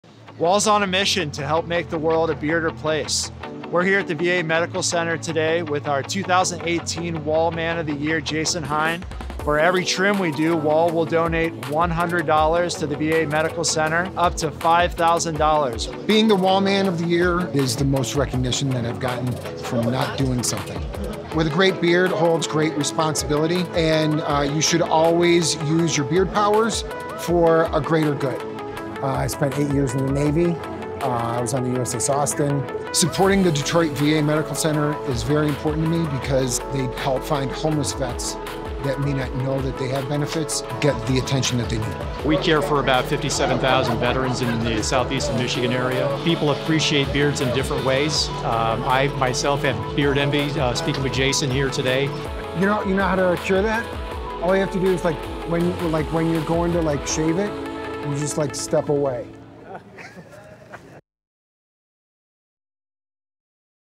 2018 ‘Wahl Man of the Year’ Uses His Facial Hair to Support Fellow Veterans
Detroit native and Navy veteran Jason Heien earned ultimate beard bragging rights when his whiskers won him the national title of ‘Wahl Man of the Year’ in 2018. Since then, Heien and his beard have been keeping busy, donating time and money to several organizations dedicated to supporting his fellow veterans — including the Detroit VA Medical Center. In honor of his good deeds, and in conjunction with Wahl’s mission to make the world a better – or bearder – place to live, Wahl parked its mobile barbershop outside the medical center on May 18, 2022. For every FREE beard trim Wahl donated $100 to support the Detroit VA Medical Center — raising $5,000 for the charity. If you’re a bearded guy who’s doing good while looking good – like Jason – you have the chance to win $20,000 for yourself, and $5,000 for your charity. Deadline to enter is June 6, 2022.